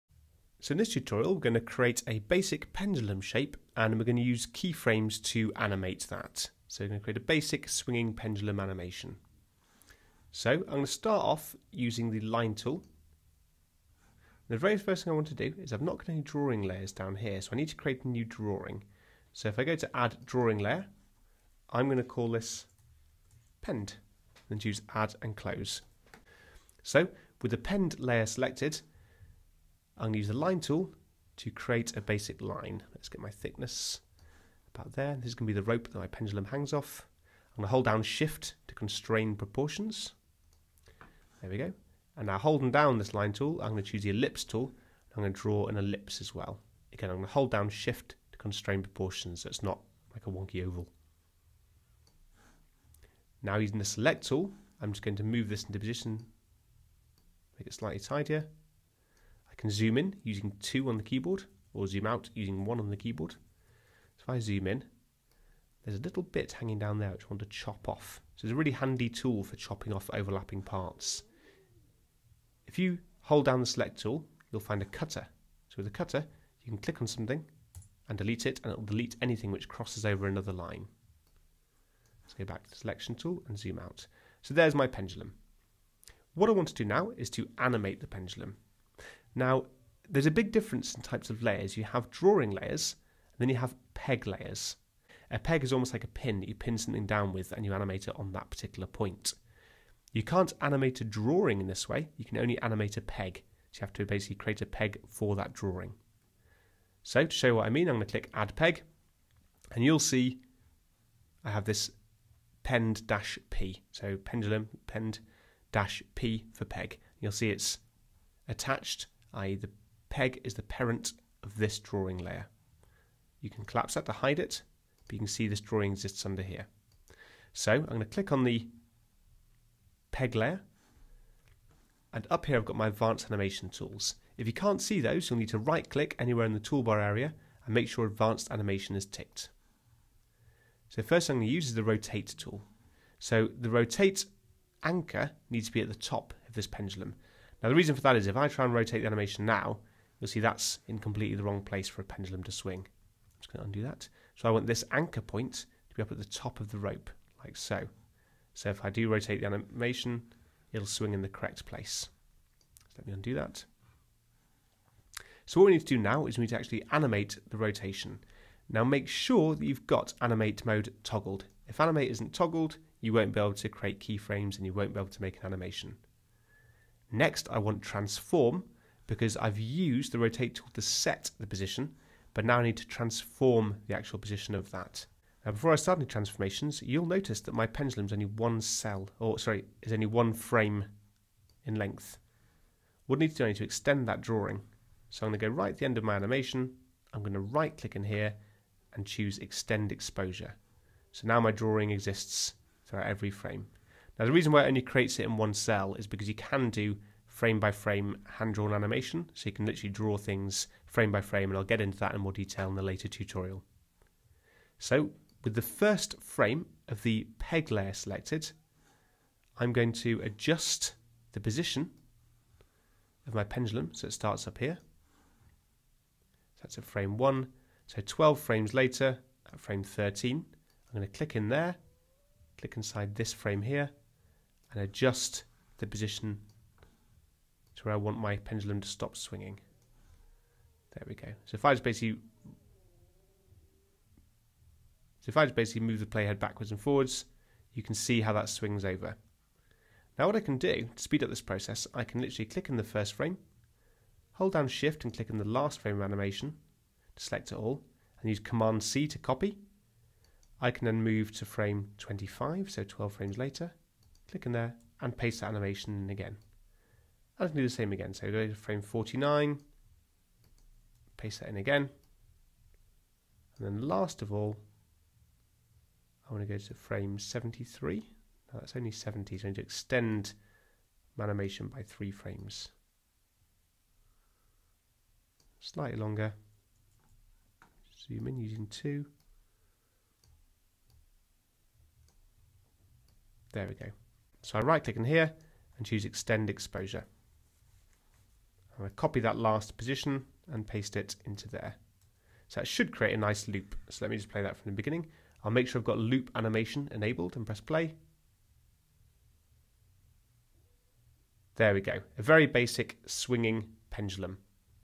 Toon Boom Harmony - Drawing a simple shape and keyframe animation
This tutorial guides you through drawing a basic pendulum, adding a peg and basic animation using keyframes.